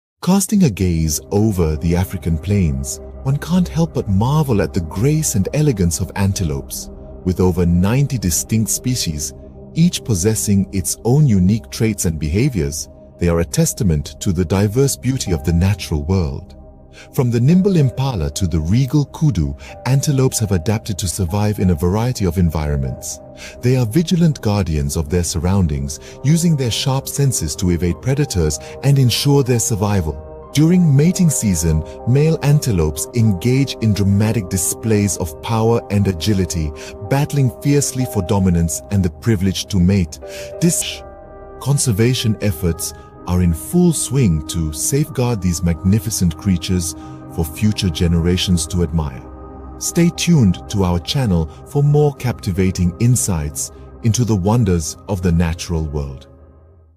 The Fascinating World of Antelopes
In this video, we will take a closer look at these graceful and majestic creatures that roam the savannas and grasslands of Africa. From the iconic impala to the elusive sable antelope, each species has its own unique characteristics and behaviors.

Join us as we delve into the world of antelopes, learning about their diet, habitat, and mating rituals. Discover why these herbivores play an important role in the ecosystem and how they have adapted to survive in the wild.

Whether you're a wildlife enthusiast or simply curious about these beautiful animals, this video is sure to captivate and educate. So sit back, relax, and let us take you on a journey through the captivating world of antelopes.